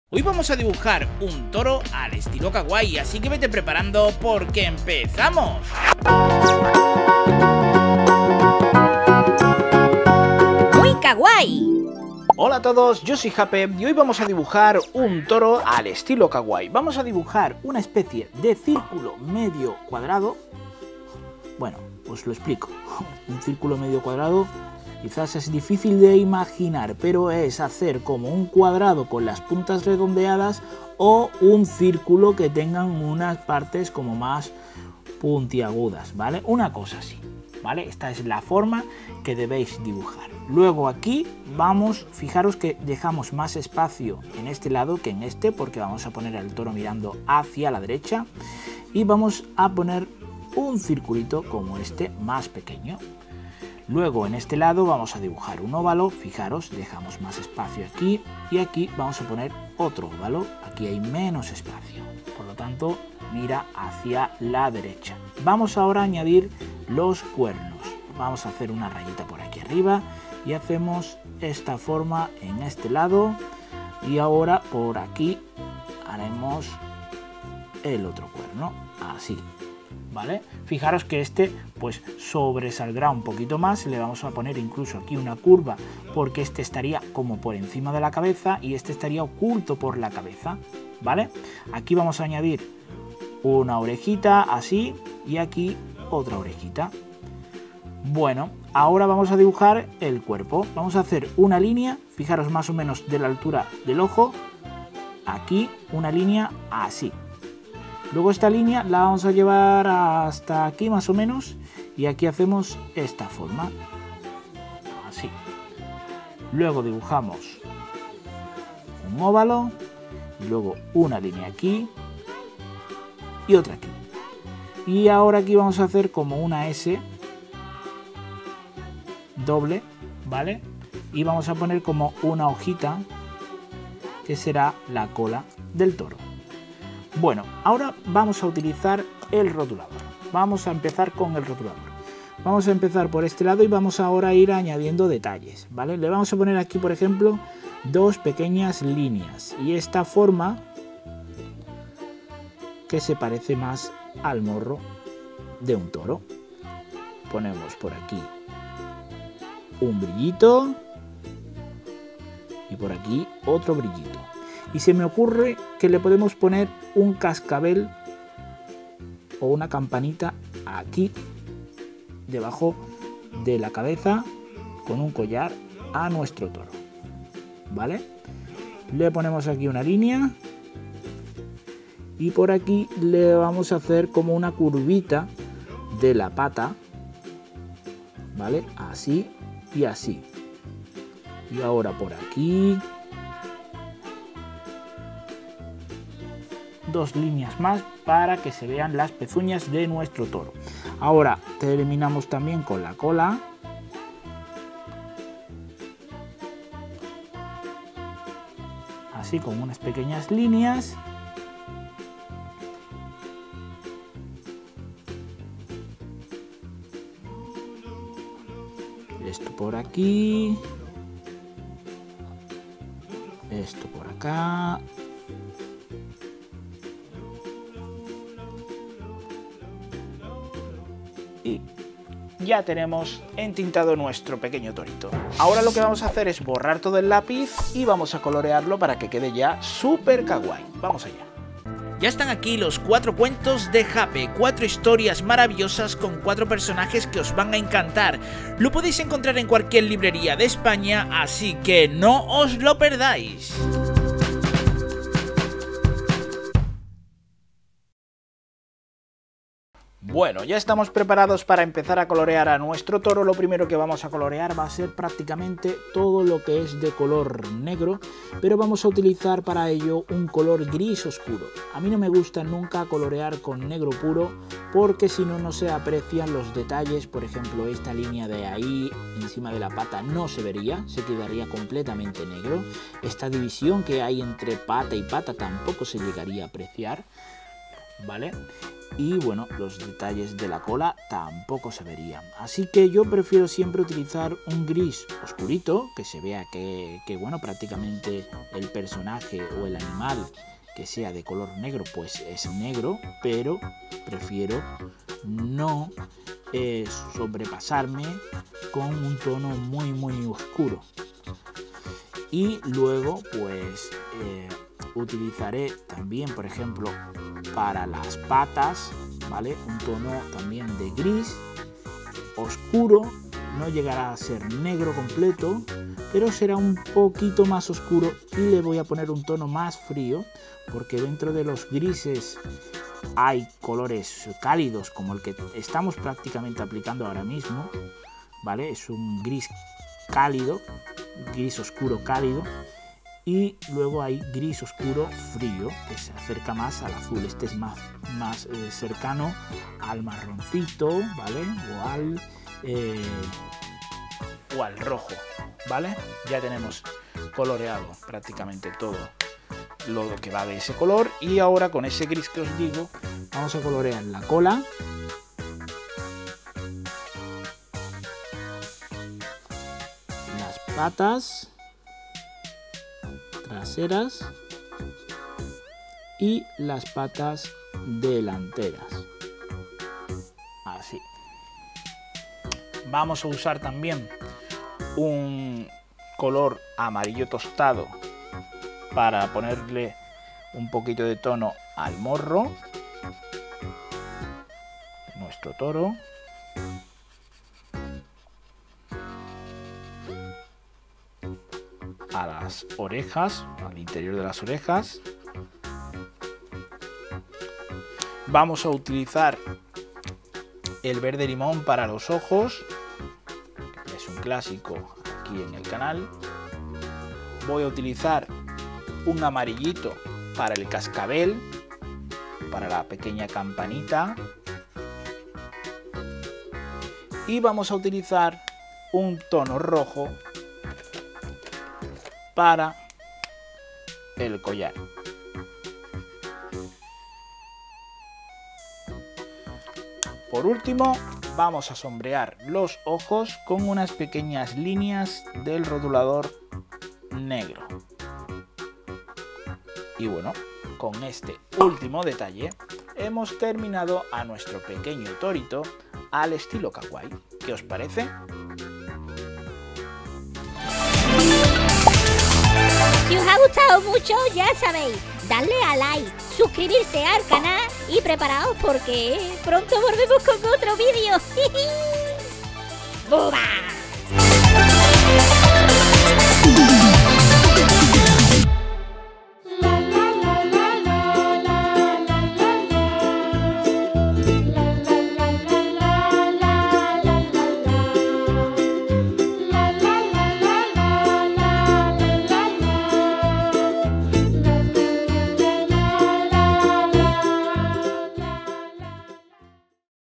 COMO DIBUJAR UN TORO - Dibujos kawaii faciles - Como dibujar animales kawaii
Como dibujar un toro kawaii es el tema de hoy, aprendera a dibujar mas animales kawaii en el canal.  Y aprende con dibujos kawaii faciles Tengo una serie de como dibujar paso a paso, animales de granja y animales salvajes de todo tipo y de una forma súper fácil.